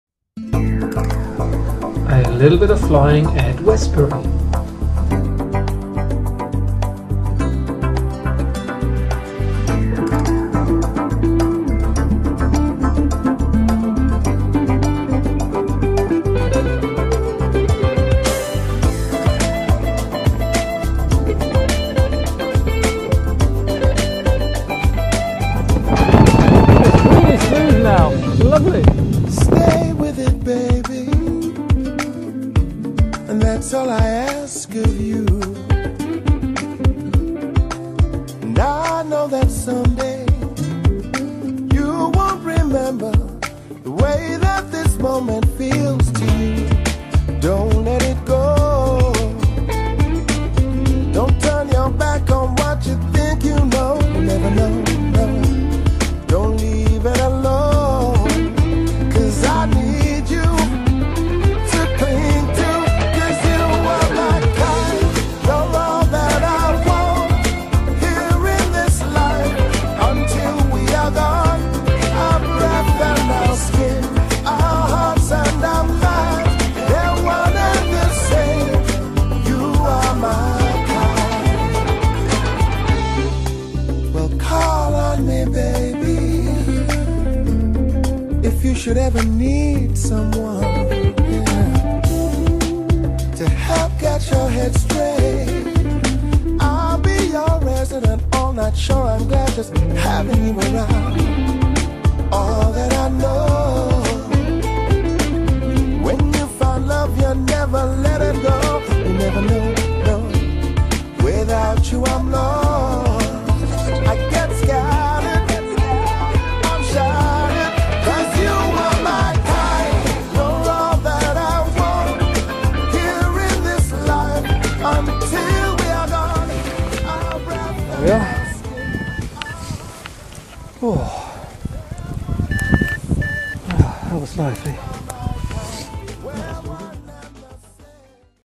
Westbury-Paragliding - Re-edit
Here is a short clip, flying at Westbury, Now I have managed to get to grips with the editing software. ;-)